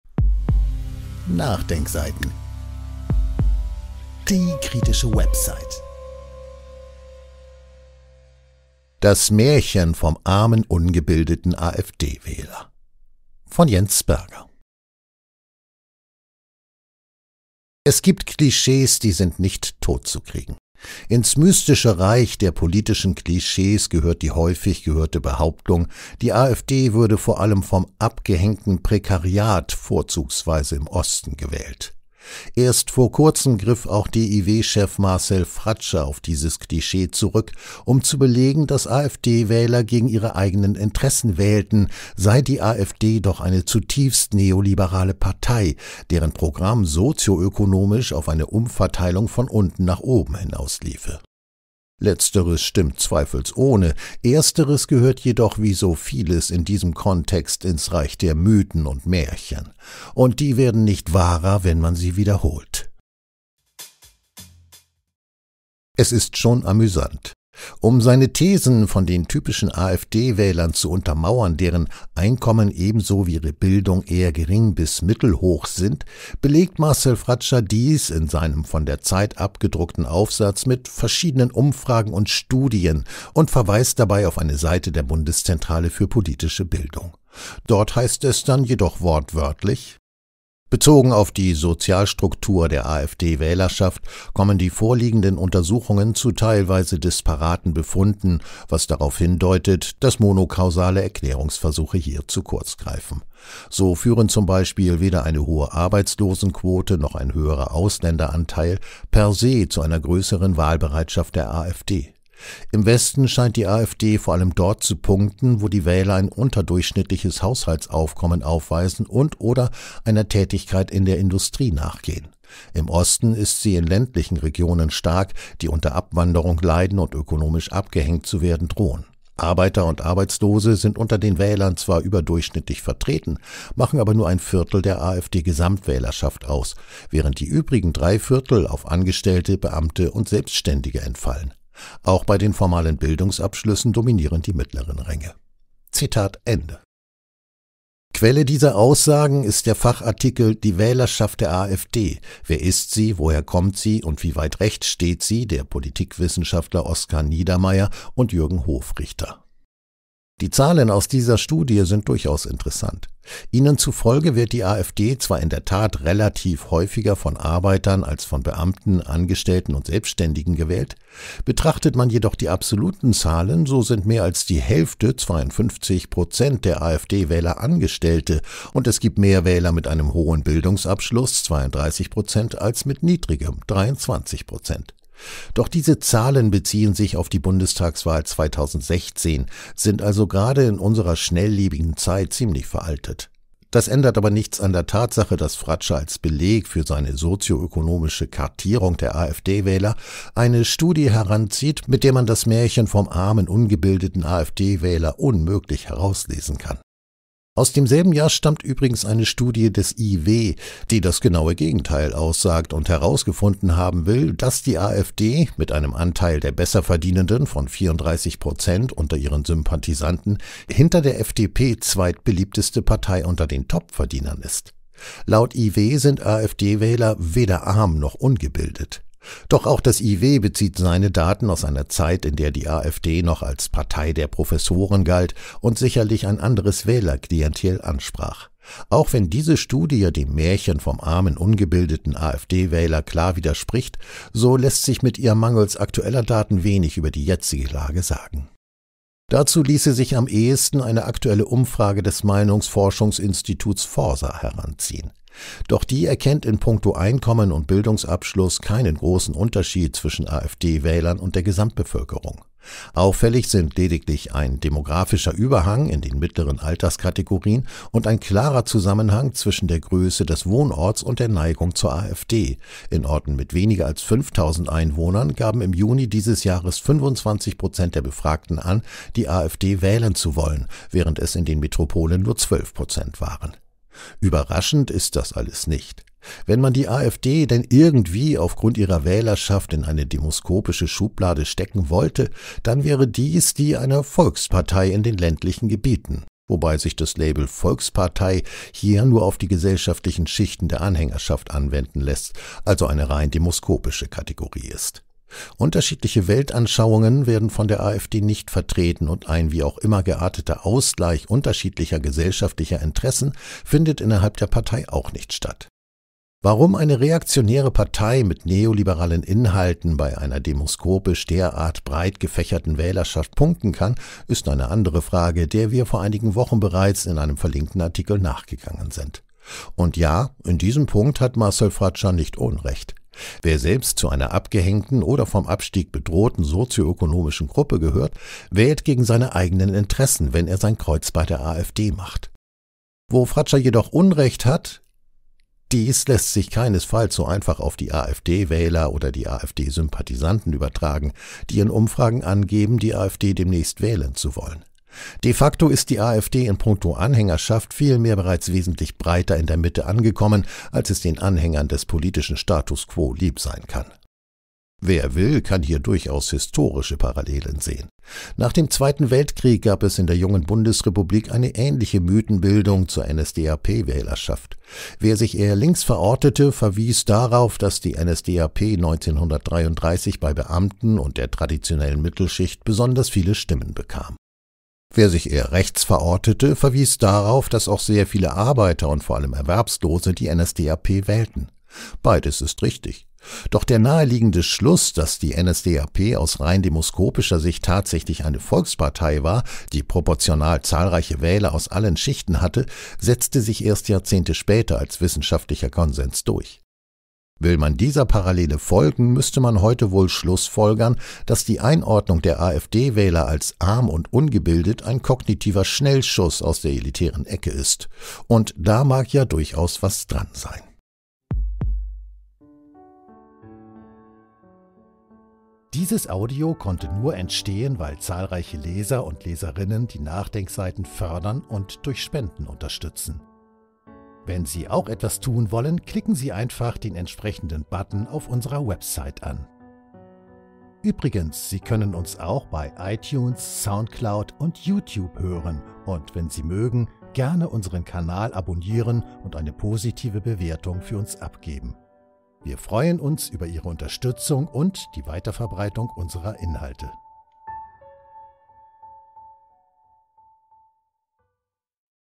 Das Märchen vom armen, ungebildeten AfD-Wähler | Jens Berger | NDS-Podcast
Das Märchen vom armen, ungebildeten AfD-Wähler | Jens Berger | NachDenkSeiten-Podcast | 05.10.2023

Es gibt Klischees, die sind nicht totzukriegen. Ins mythische Reich der politischen Klischees gehört die häufig gehörte Behauptung, die AfD würde vor allem vom „abgehängten Prekariat“ – vorzugsweise im Osten – gewählt. Erst vor kurzem griff auch DIW-Chef Marcel Fratzscher auf dieses Klischee zurück, um zu belegen, dass AfD-Wähler gegen ihre eigenen Interessen wählten, sei die AfD doch eine zutiefst neoliberale Partei, deren Programm sozioökonomisch auf eine Umverteilung von unten nach oben hinausliefe. Letzteres stimmt zweifelsohne, Ersteres gehört jedoch wie so vieles in diesem Kontext ins Reich der Mythen und Märchen; und die werden nicht wahrer, wenn man sie wiederholt. Von Jens Berger.

Artikel veröffentlicht am: 05. Oktober 2023 um 10:00
Titelbild: knipsdesign/shutterstock.com (Chemnitz, Germany - September 01, 2018: Afd demonstration Trauermarsch)

AfD, Kampagnen, Tarnworte, Neusprech, Bundeszentrale/Landeszentrale für politische Bildung, Forsa, Marcel Fratzscher, IW, sozioökonomischer Status, Volkspartei